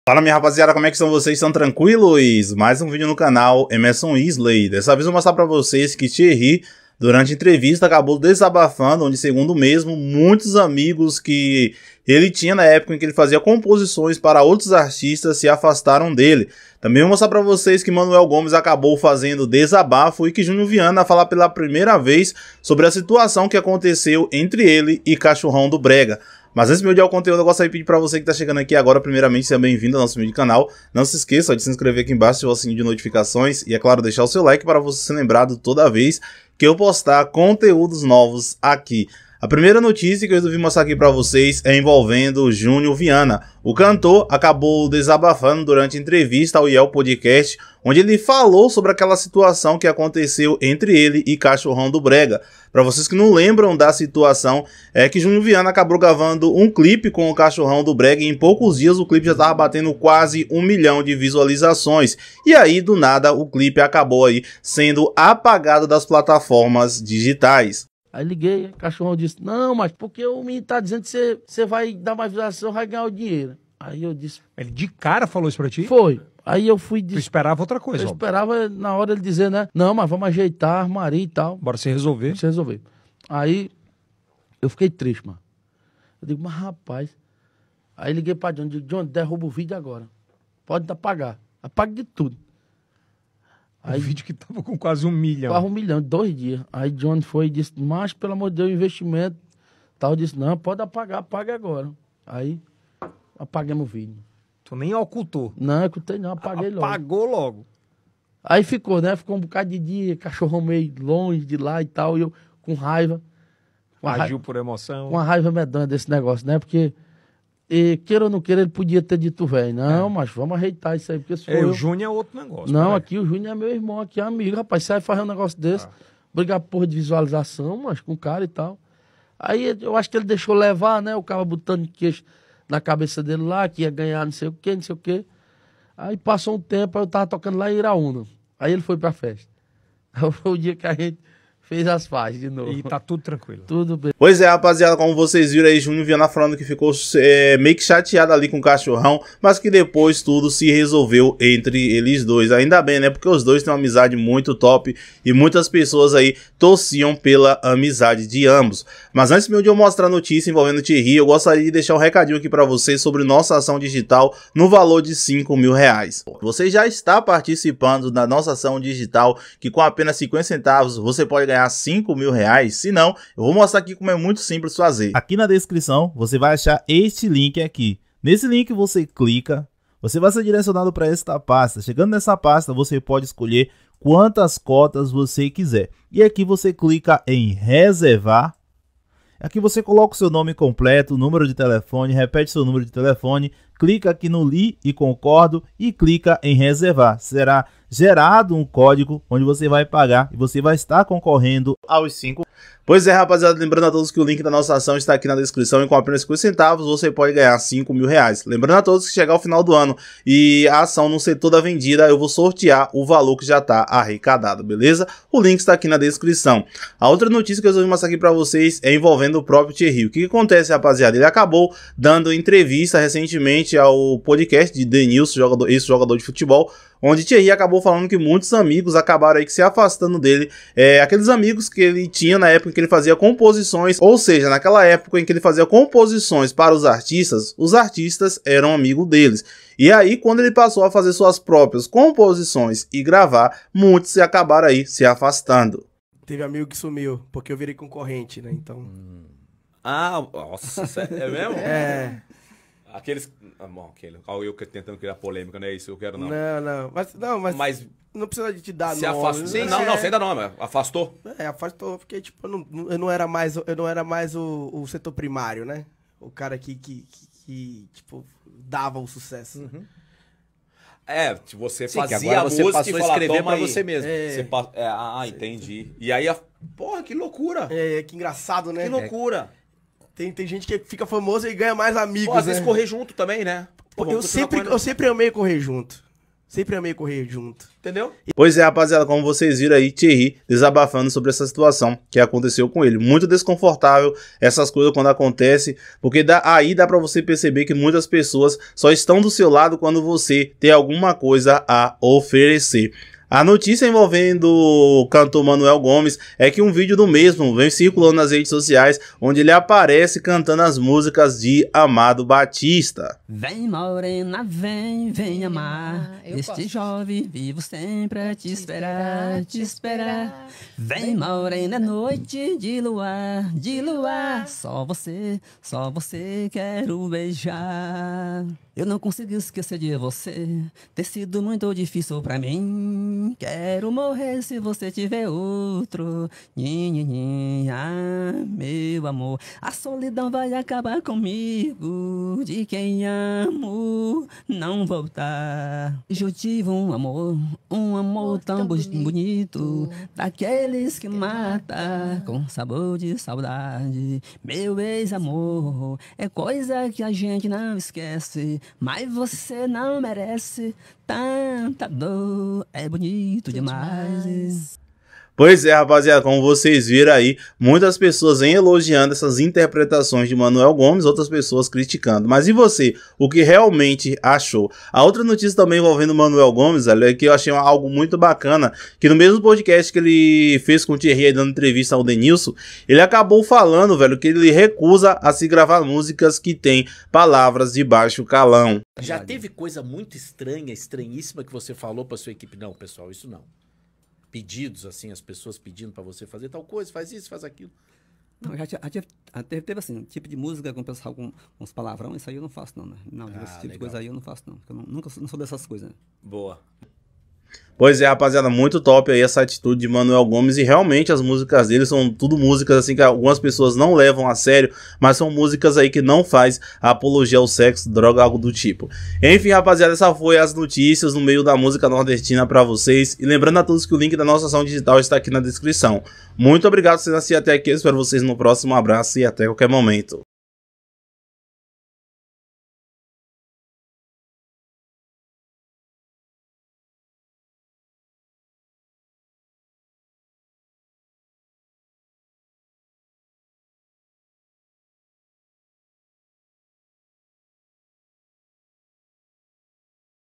EM DESABAFA TIERRY FALA QUE MUITOS AMIGOS SE AFASTARAM- MANOEL GOMES DESABAFA-VIANNA FALA DE...
TIERRY FALA DOS AMIGOS QUE SE AFASTARAM DELE, JUNIOR VIANNA FALA DE CACHORRÃO DO BREGA MANOEL GOMES DESABAFA...